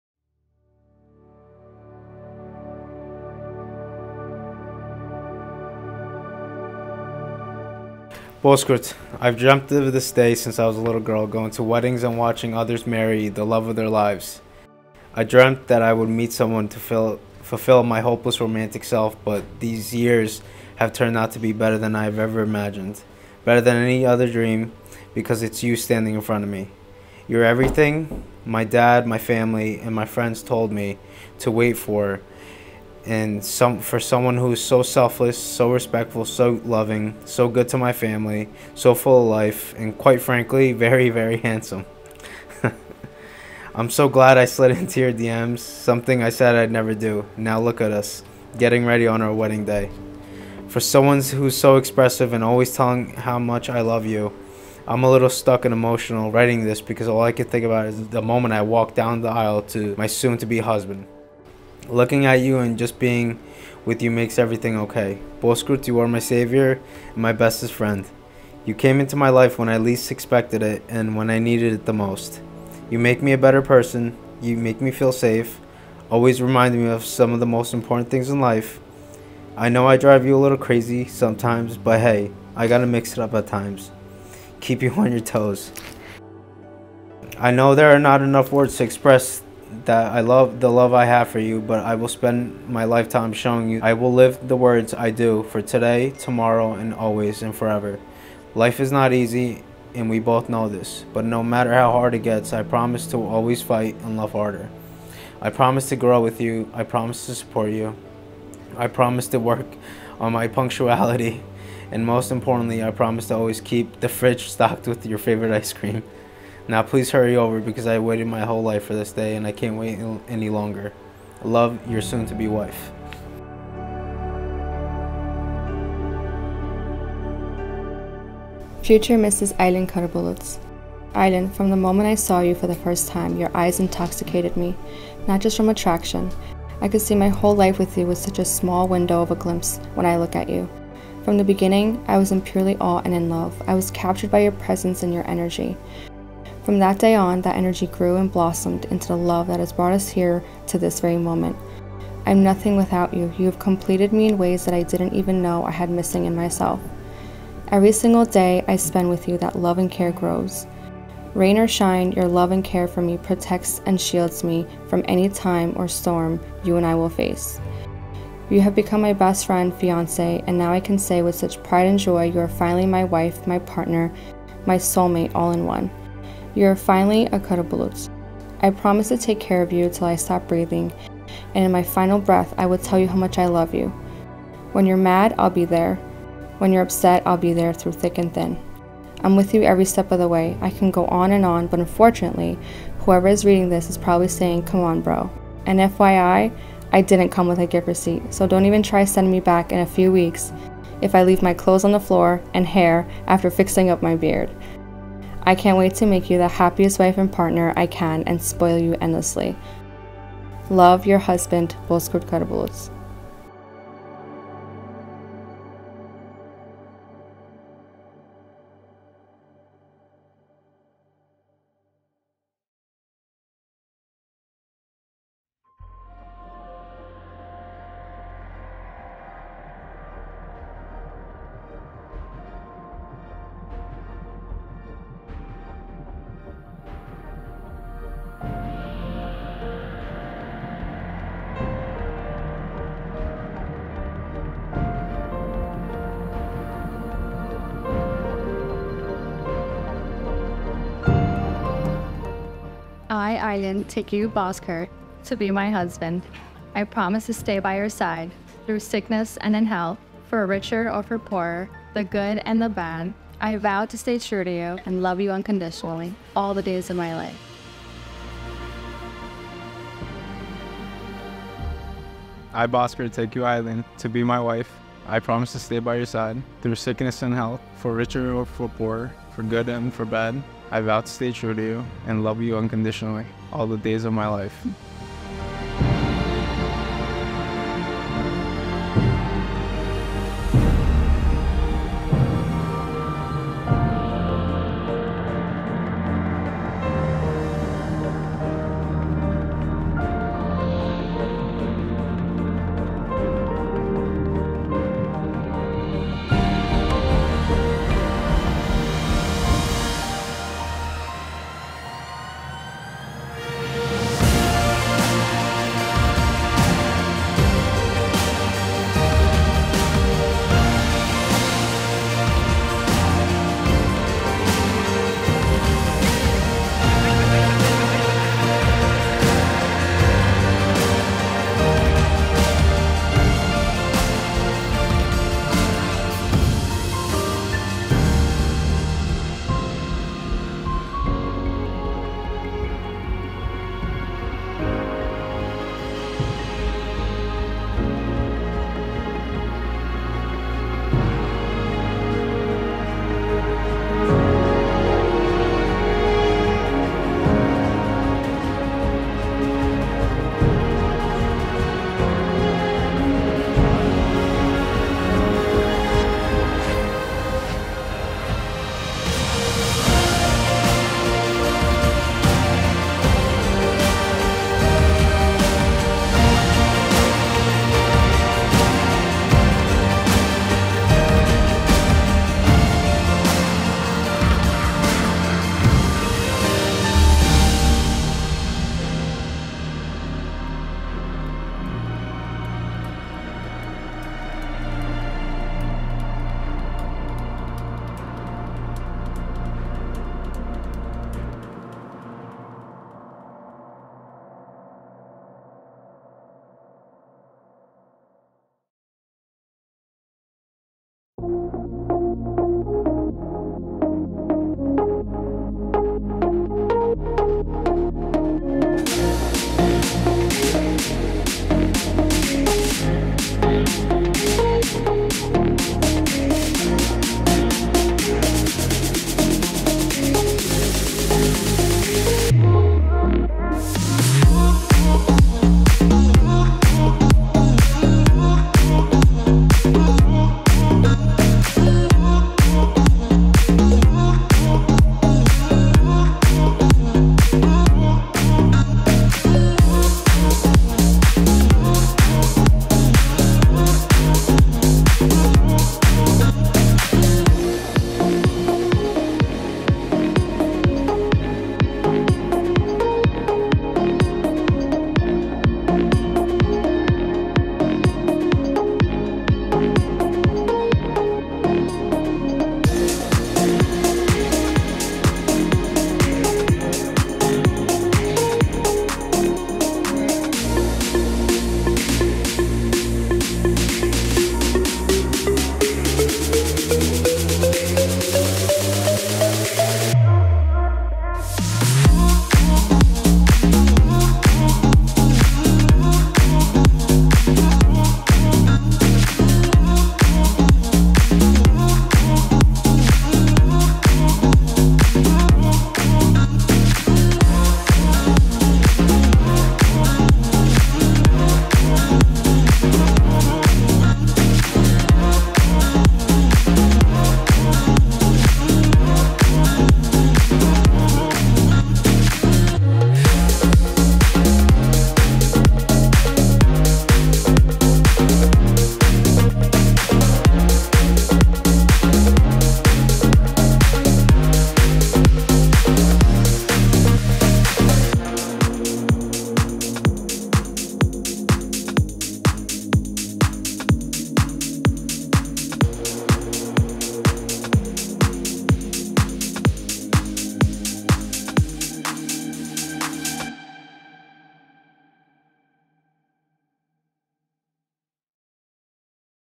Aylin & Bozkurt Wedding Highlight 5/5/23
✨CONGRATULATIONS to THE NEW Mr. & Mrs. Karabulut✨Aylin and Bozkurt’s Wedding Day 5/5/23 - The Venetian - Garfield, NJ - Ralph DePiro Photography 📸
To start off our wild wedding weekend, Wedding 1/3 we had the pleasure of shooting Aylin and Boz’s wedding day on 5/5/23 - Cinco De Mayo! Now, we all know Cinco De Mayo is a good time, and let me tell ya, it did not disappoint! Weddings on Cinco De Mayo, just double the fun. We had met Aylin and Boz at a friends wedding back in October and immediately hit it off.
They were guests that night and saw the RDP Team in action. When the day was coming to an end, they approached us with such excitement and discussions about their wedding plans. We set up a time to chat, talked about their wedding day, and the rest is history!
Let’s take a look at the NEWWW, Mr. & Mrs. Karabulut💍!
🏠Venue: @thevenetiannj
📸Photo: @ralphdepirophotos
🎥Video: @ralphdepirophotos
💇🏻‍♀️Hair: @anahellabridal
💄Make up: @pixydust
👗Dress: @sengcoutureofficial
💍Rings: @kentnewyork
🎤DJ/Band: @magicmomentsnj
🎂 Cake: @palermos.bakery
💐 Florist: @bloombywedded
💌 Invitation: @truly.engaging
🥁Drummer: @mabrookentertainment